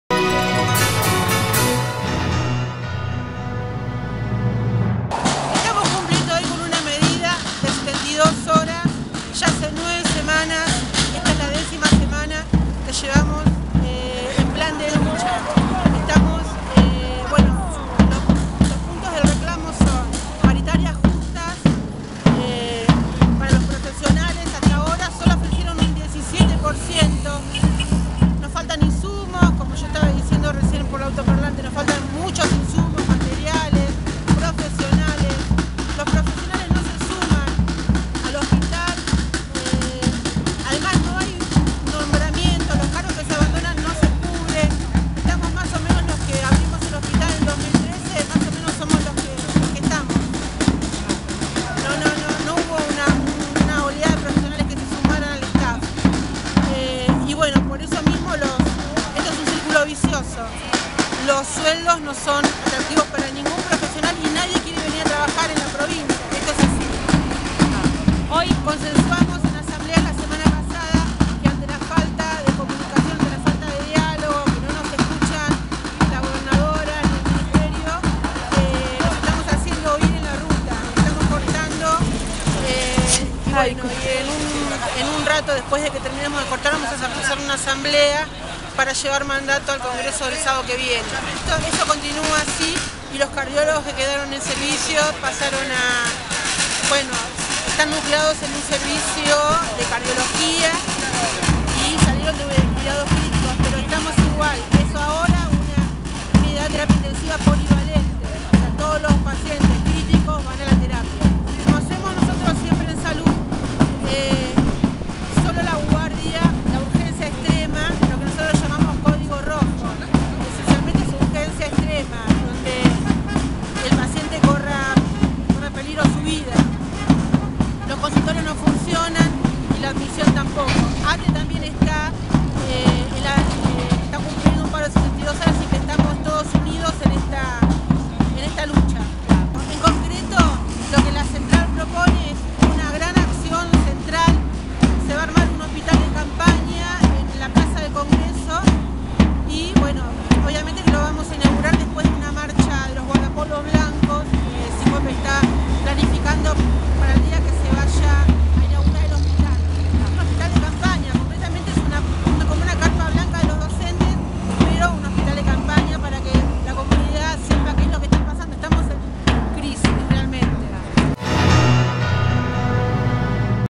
Trabajadores del Hospital Balestrini se manifestaron sobre Camino de Cintura
El tránsito se vio interrumpido en horas del mediodía por un nuevo reclamo de los empleados del nosocomio de Ciudad Evita.